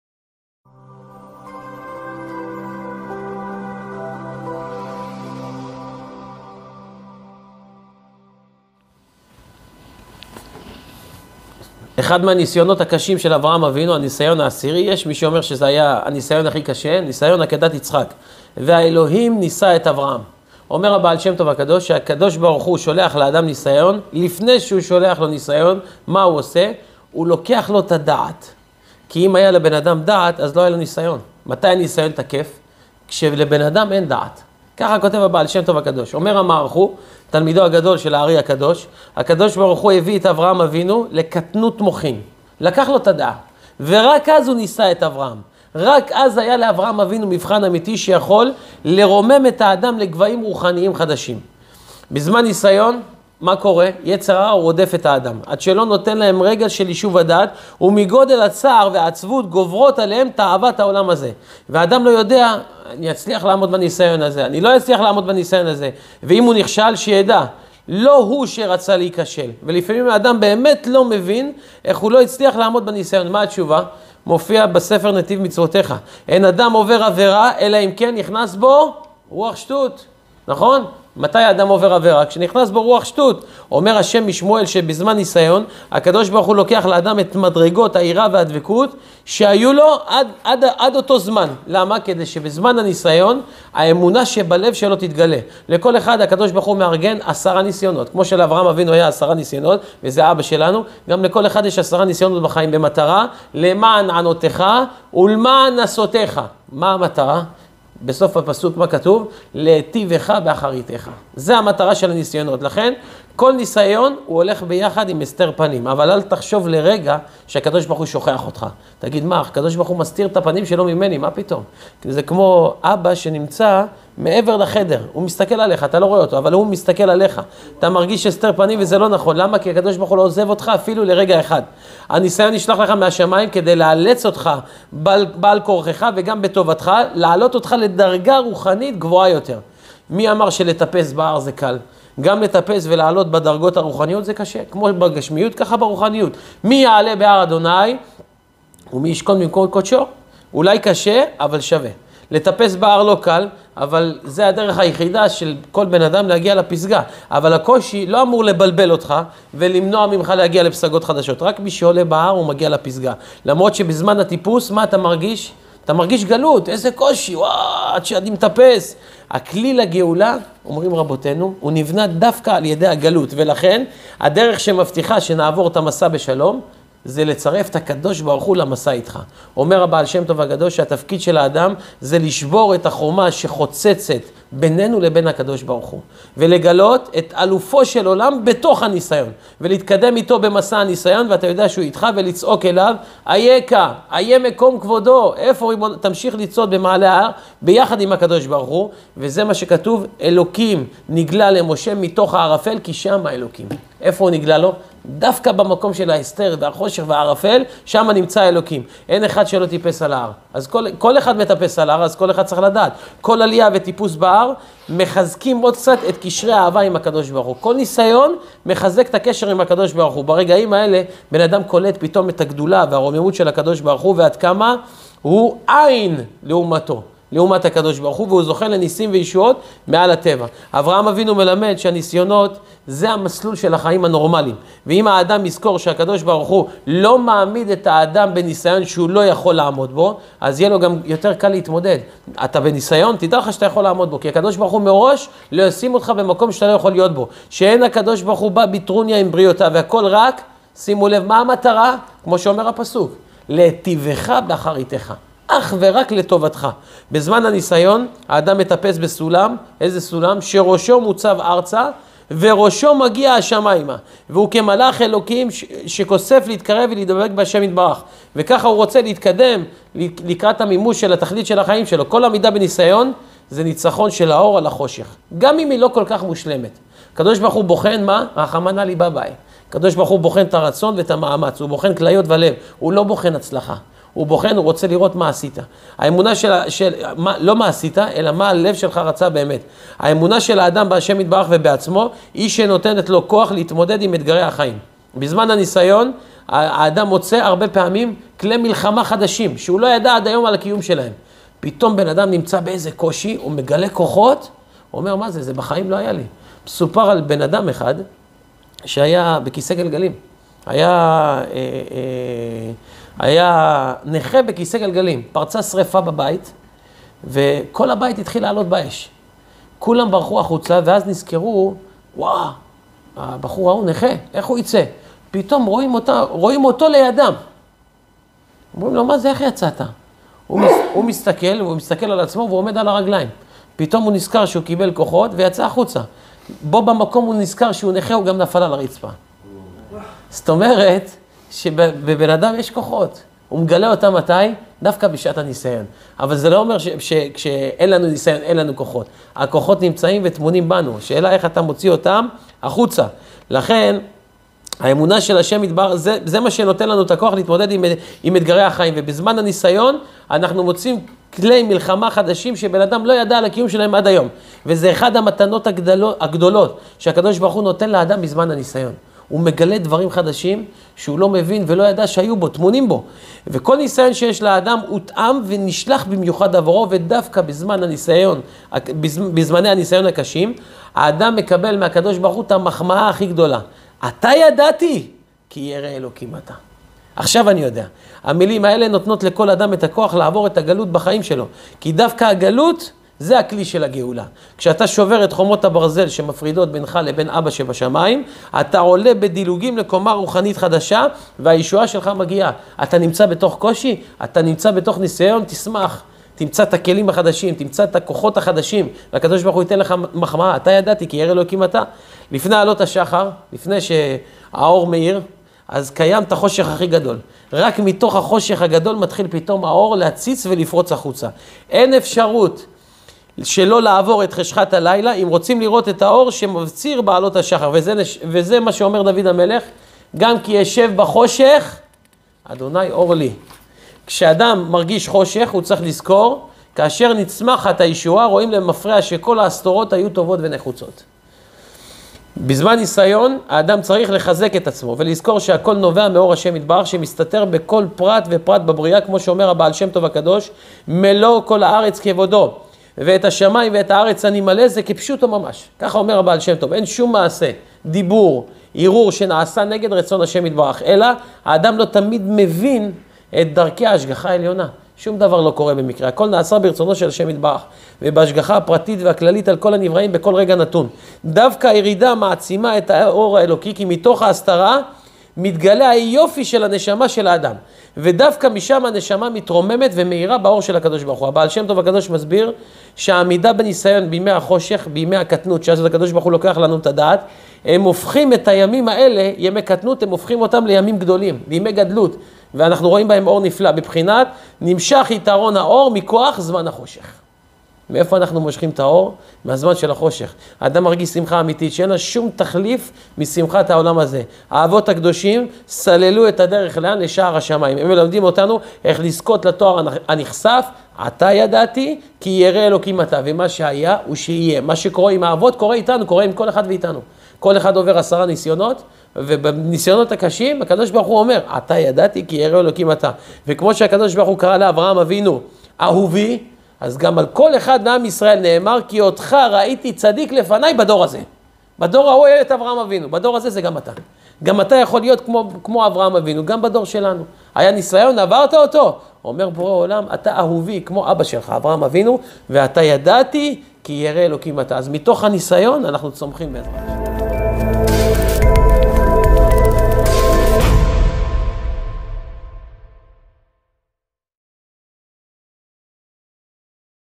אל תחליק! זה רק נסיון! 💫 מטרת הנסיון להטיבך באחריתך! 💫 הגלות הכלי לגאולה💫לימוד מפרשת השבוע💫פרשת וירא
מדברי התורה הנפלאים של הר, ניסים חורפיש שיח'

״אני באתי לעולם הזה להראות דרך אחר. והוא, שיראה האדם להמשיך על עצמו שלשה דברים הללו, היינו: אהבת ה׳ ואהבת ישראל ואהבת התורה, ואין צריך לעשות סיגופים״ – כך סיפר הרב הקדוש מוהר״ר ברוך ממז׳בוז׳ בשם זקנו הבעל שם טוב זיע״א

הבעל שם טוב הקדוש, האיש הגדול בענקים, שמע תורה לא מפי מלאך ולא מפי שרף אלא מפי הגבורה ממש (דגל מחנה אפרים, ליקוטים) ״כשהיה צריך לדעת שום דבר פרט, אפילו מעניני בני אדם, דבר גשמי, היה מסתכל בהתורה ולמד בה בדחילו ורחימו ומדבק את עצמו, עד שהגיע לאור הגנוז בתוכה. והיה רואה בדבר שצריך להיות, ממש כמו שכתוב בש״ס מסוף העולם ועד סופו (מאור עינים). הוא הגבר אשר לימודו, כאשר יתפרסם בעולם יבוא משיח צדקנו במהרה (אגרת הבעש״ט שנדפסה בסוף ספר פורת יוסף, מהדורה ראשונה)

מתורתו המופלאה של הבעל שם טוב הקדוש, נערך ע"י הרבנית שאלתיאל שתח'.  - 'להגביה עוף'